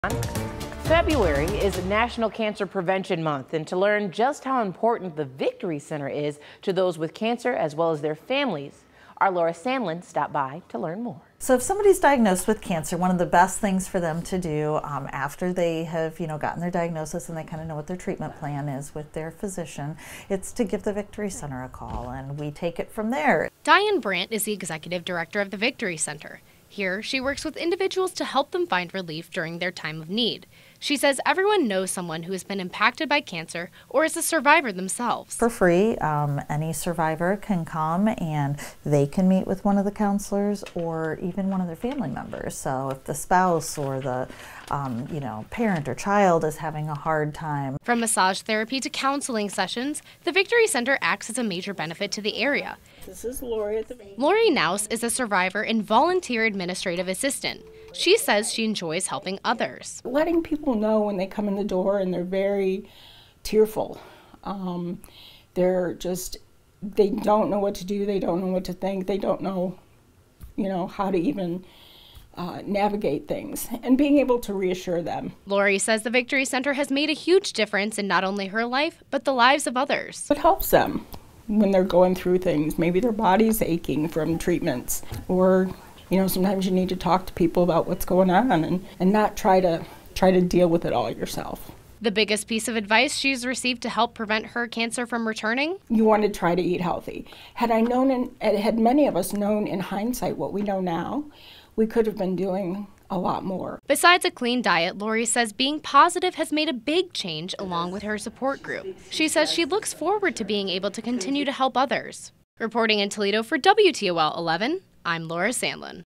Victory Center offers counseling, support, other resources to newly diagnosed cancer patients
February marks the start of National Cancer Prevention Month.

Whether you're just beginning your fight, or you're in remission, it's important to know where you can get support.

One of the places that people turn to in Toledo is the Victory Center, a non-profit that offers wellness services for cancer patients at no-cost.

The Victory Center is partnered with both Mercy and ProMedica, and together, they service the local area, helping people who have been newly diagnosed with cancer.